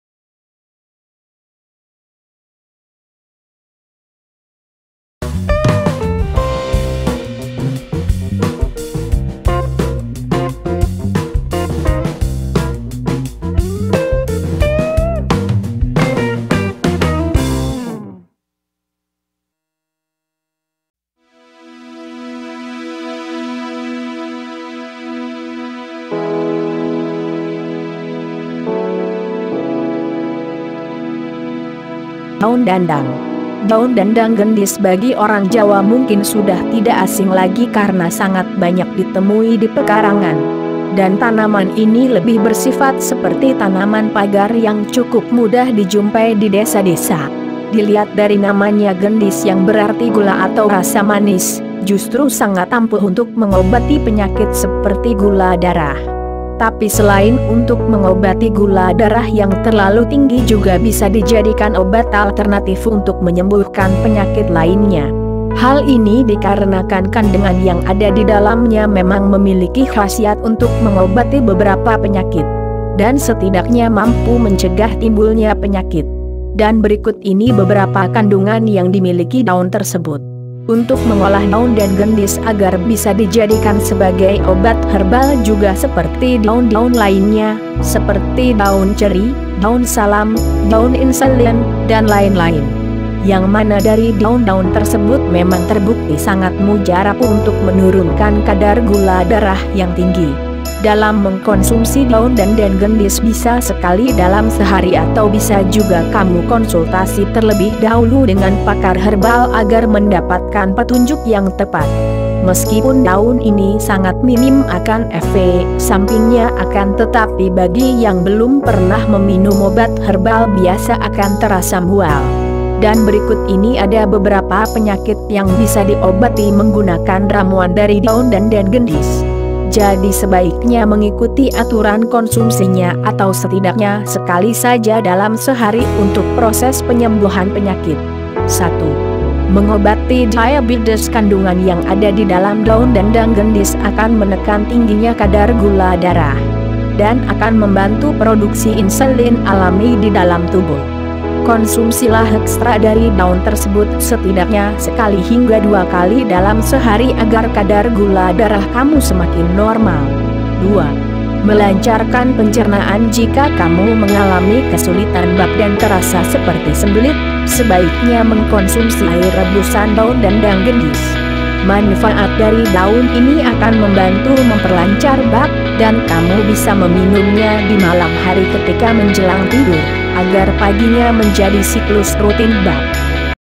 TAK TERDUGA!!! TERNYATA TANAMAN DANDANG GENDIS SANGAT BERMANFAAT UNTUK OBAT DIABETES
Untuk mengolah daun dan gendis agar bisa dijadikan sebagai obat herbal juga seperti daun-daun lainnya, seperti daun ceri, daun salam, daun insulin, dan lain-lain. Yang mana dari daun-daun tersebut memang terbukti sangat mujarab untuk menurunkan kadar gula darah yang tinggi.